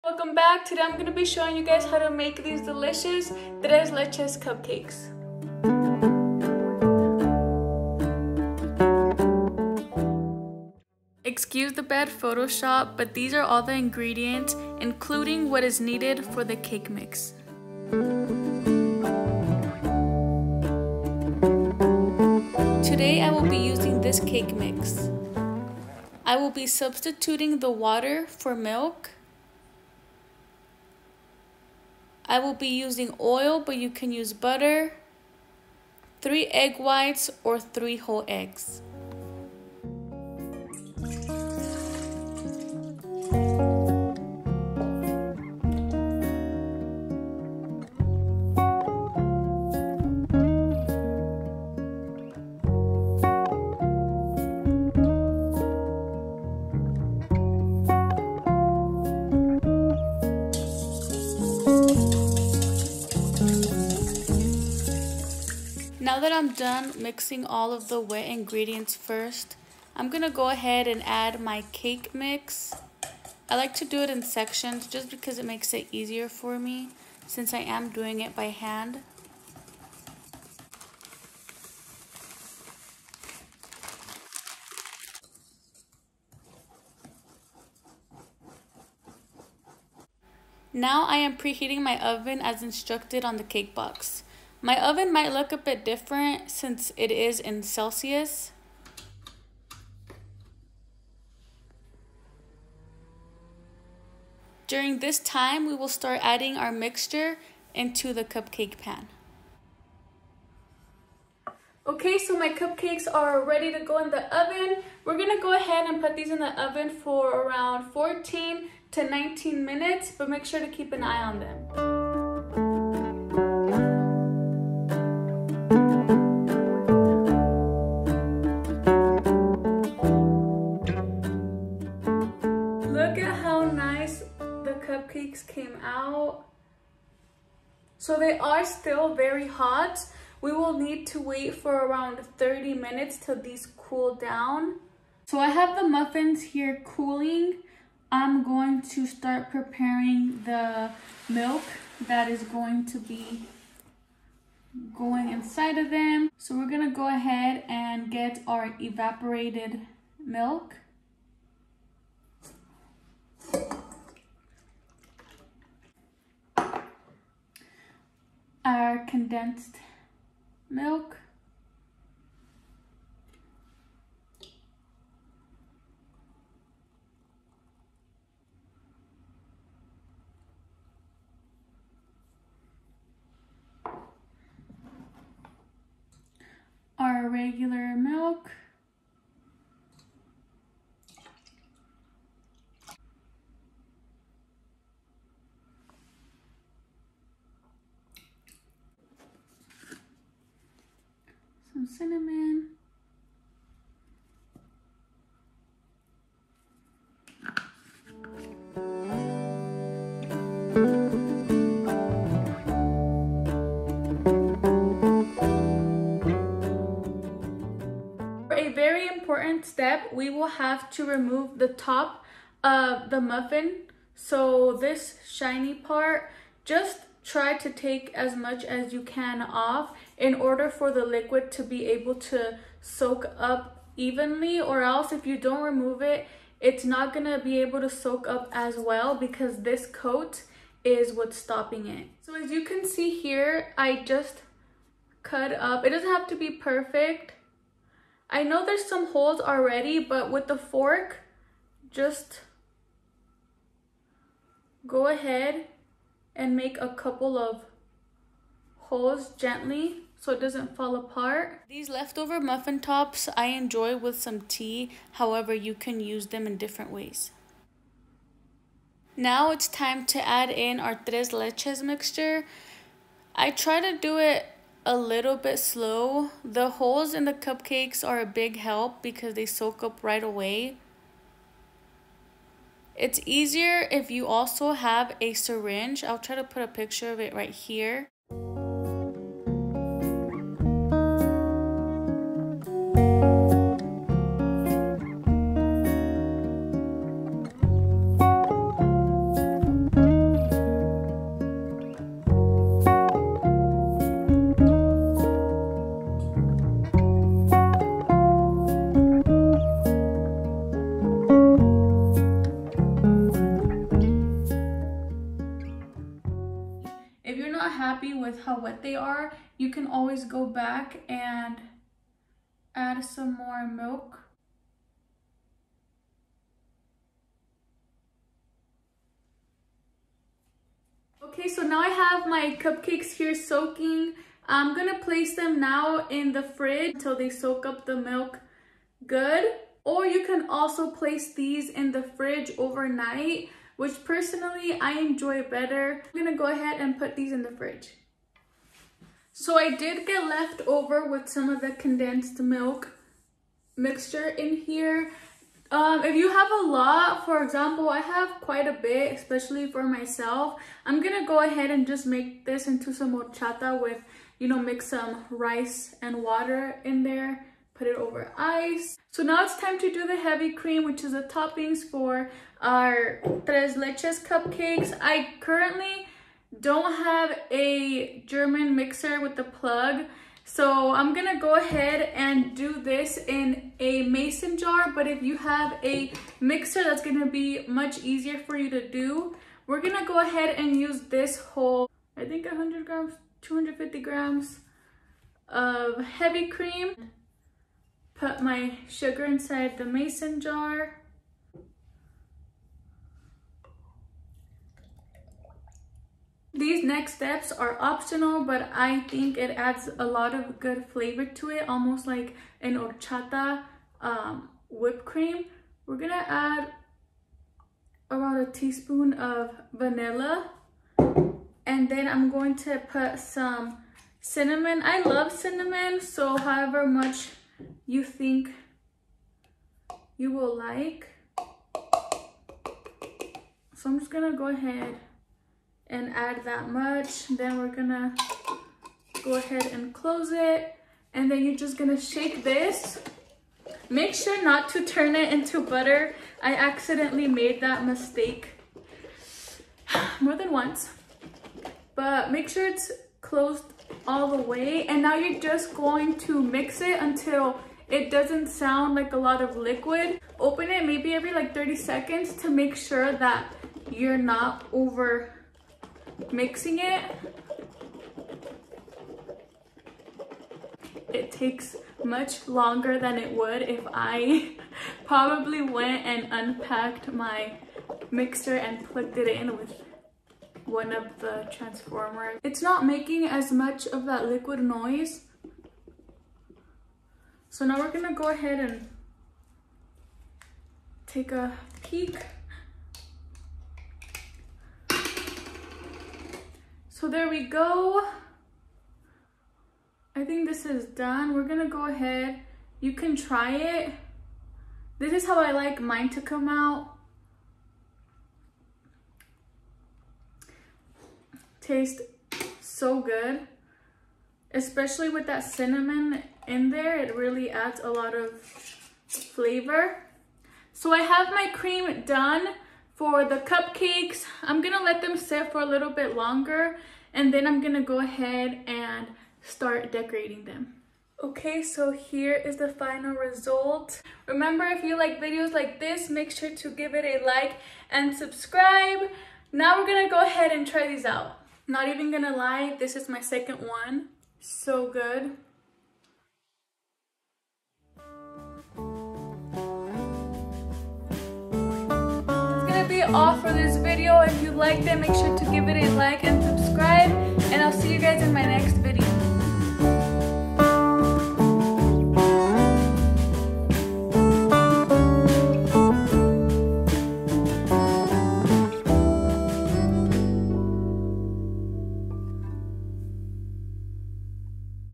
Easy and Delicious Tres Leches Cupcake Recipe
Hope all is well ! Thank you for the support and very kind messages. If you have any more questions feel free to message me and I will make more of these recipes for you !

Things needed:
-1 can Evaporated milk
- 1 can Sweetened condensed Milk
-Total of 2 cups of milk ( 1 for CAKE MIX, the other for TRES LECHES MIXTURE )
- Heavy Cream
-  1 Cake mix box
-1/2 Cup butter or oil
-3 eggs
- Cinnamon
- 3 tea spoons regular sugar
- Vanilla

TOPPINGS
-Strawberries